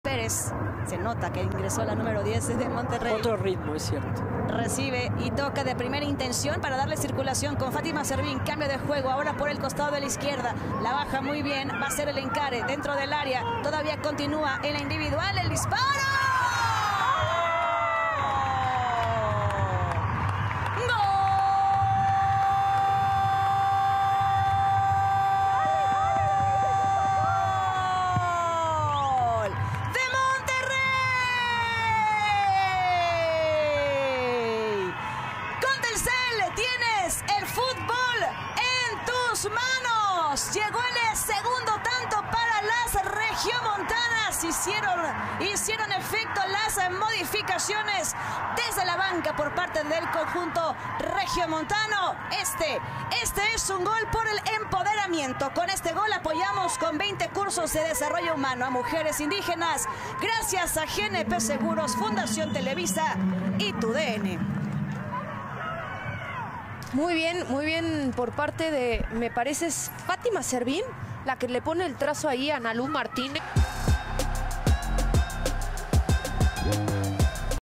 ¡Ana Lucía se lució con un golazo! | Pumas 0-2 Monterrey | Liga Mx Femenil AP2024-J15 | TUDN
La futbolista entró de cambio y se hizo presente en el marcador.

La mayor oferta de partidos de la Liga Mx Femenil en ViX